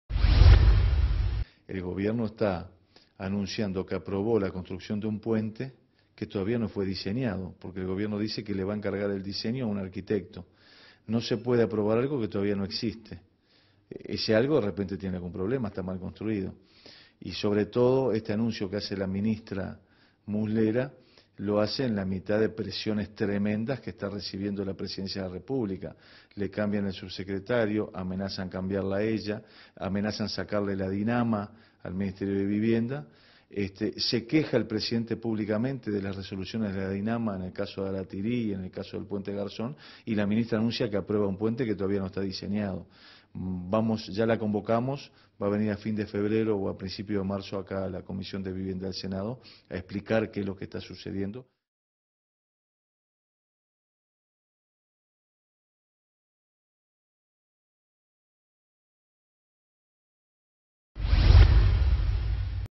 Oposición convoca a Muslera
El Gobierno anunció la autorización de la  construcción del polémico puente sobre la Laguna Garzón, que separa los departamentos de Maldonado y Rocha.
La decisión, que en los últimos años generó una fuerte controversia entre empresarios argentinos que tienen propiedades en la zona, fue anunciada en una rueda de prensa por el secretario de la Presidencia, Alberto Breccia.
Según Breccia, para la construcción del puente el Ejecutivo ha tocado la puerta del arquitecto Rafael Vignoli, el responsable de la obra del Aeropuerto Internacional de Carrasco (AIC), en Montevideo.
Como parte del proyecto, el Gobierno convertirá la carretera que va de José Ignacio hasta La Paloma, en Rocha, de ruta nacional a camino departamental "para evitar el tránsito por allí de camiones y maquinaria pesada".
La senadora oficialista Mónica Xavier opinó que para la construcción del puente "hay que ver los estilos que se apliquen en el lugar y que vayan en la misma línea del paisaje del lugar".
Desde la oposición, el senador colorado Pedro Bordaberry dijo que "no se puede aprobar algo que todavía no existe. Aún no se sabe cómo va a ser la construcción", aseguró.
Los colorados convocaron a la ministra de Vivienda, Graciela Muslera, a la Comisión respectiva del Parlamento.